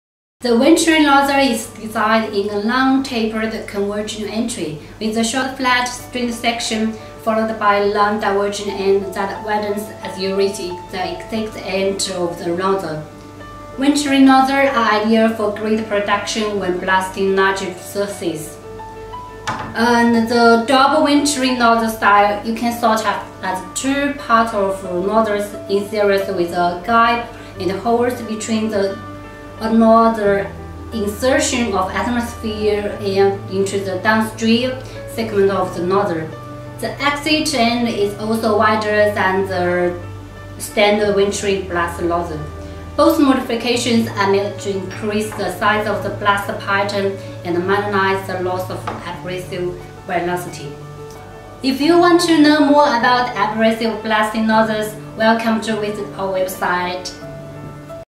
This is Venturi Nozzle
How much do you know about the venturi nozzles? Do you know the differences between single venturi nozzles and double venturi nozzles?

👇This video is going to talk about the venturi nozzle in detail.